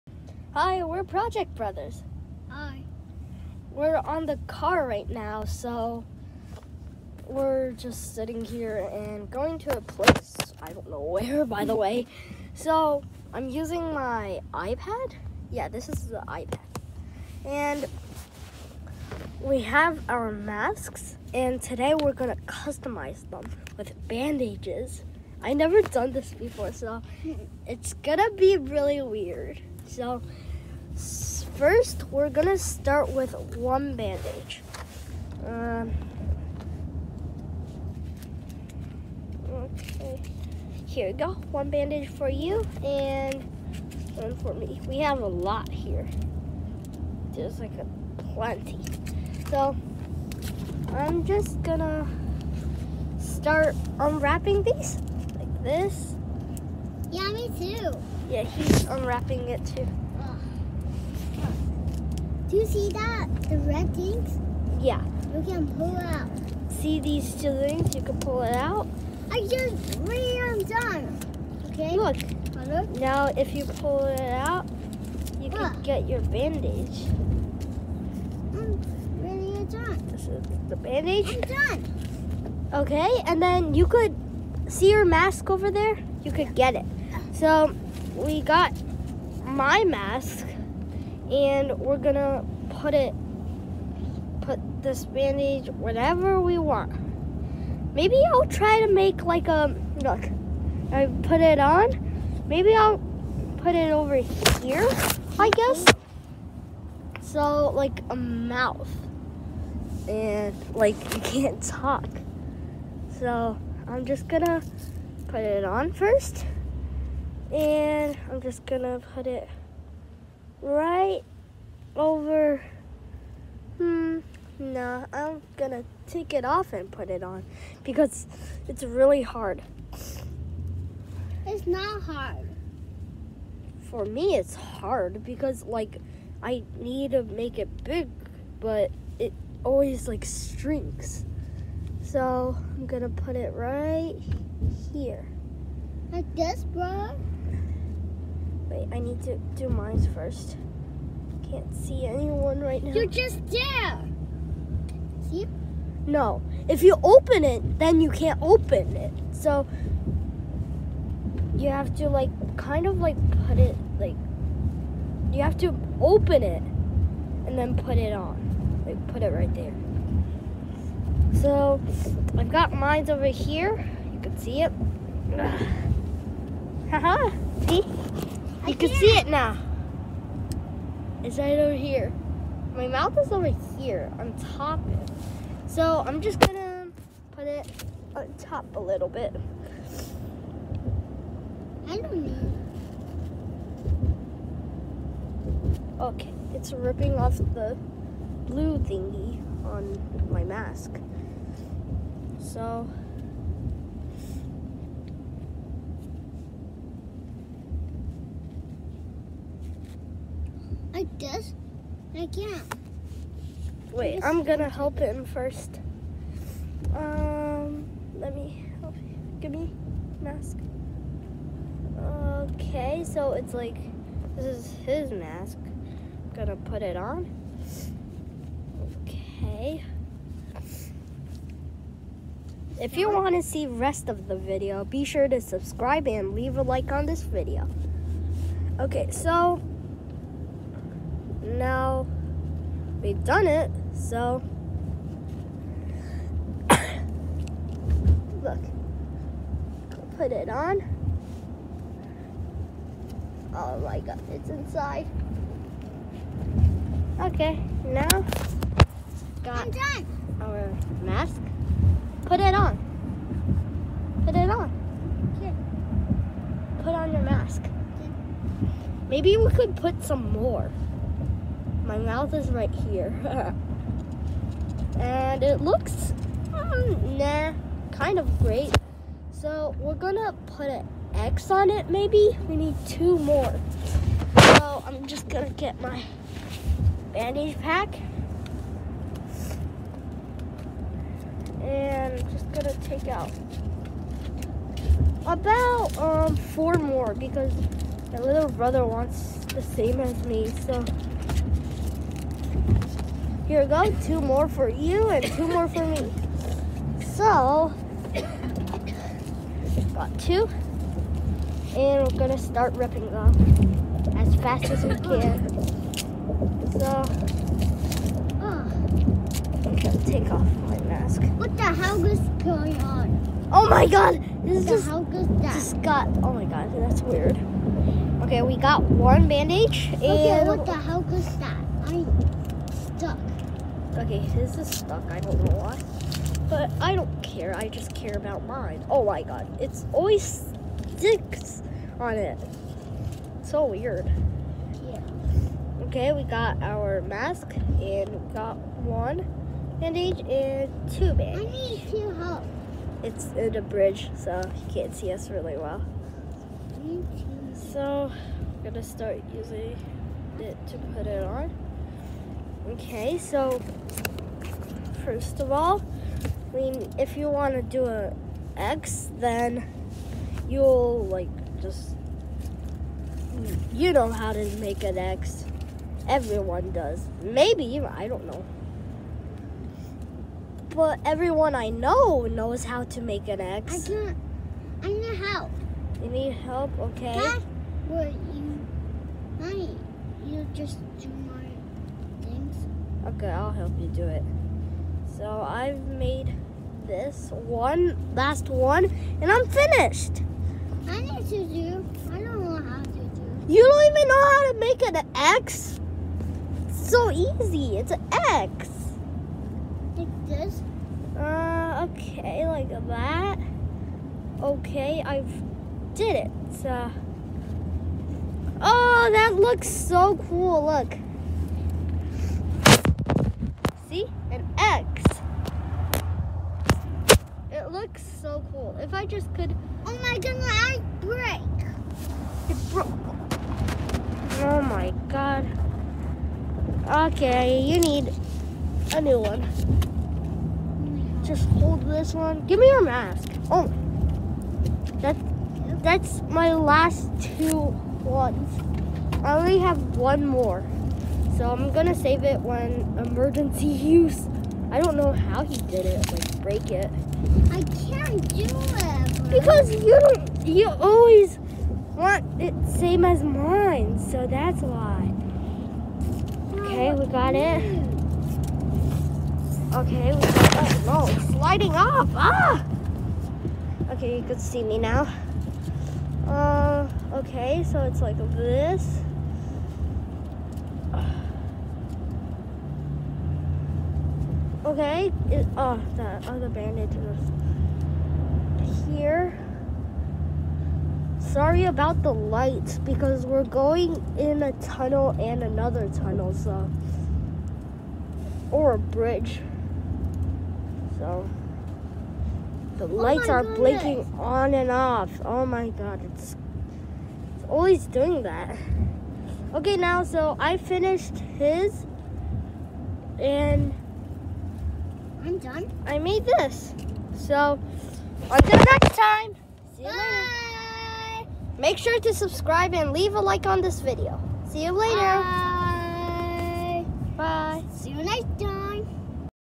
Designing face masks
Today we’re going to design face-masks it will be really fun let’s try it out